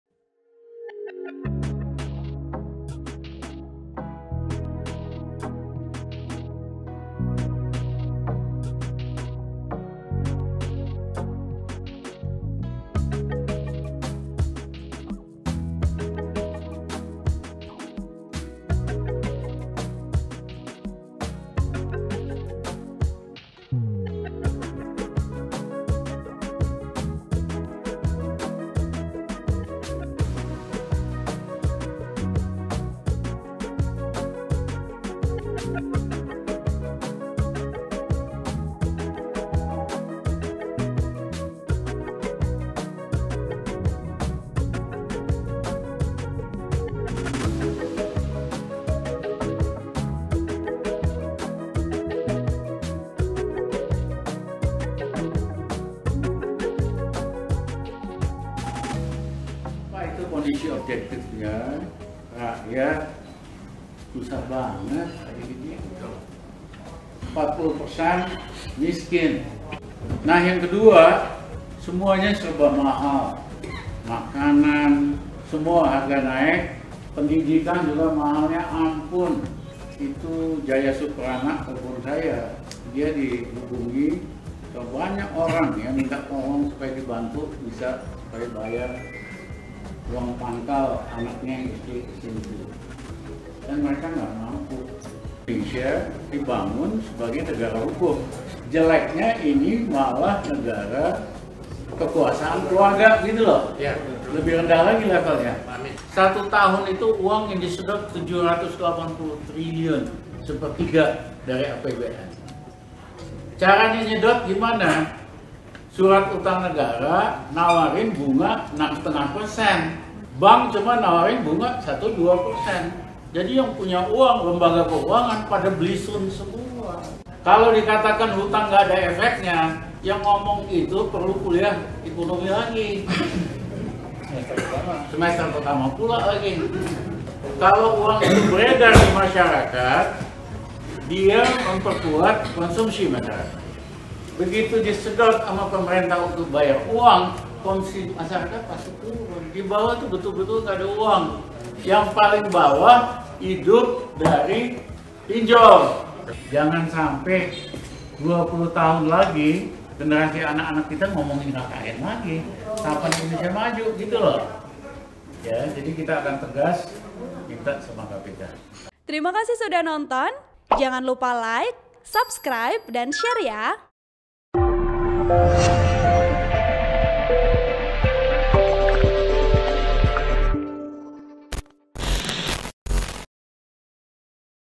Ekonom Rizal Ramli Sebut Kondisi Perekonomian Indonesia Era Jokowi Tidak dalam Keadaan Baik
TRIBUN-VIDEO.COM - Konidisi perekonomian Indonesia di zaman kepemimpinan Presiden Joko Widodo (Jokowi) tidak dalam keadaan baik, terutama semenjak pandemi Covid-19 hingga saat ini.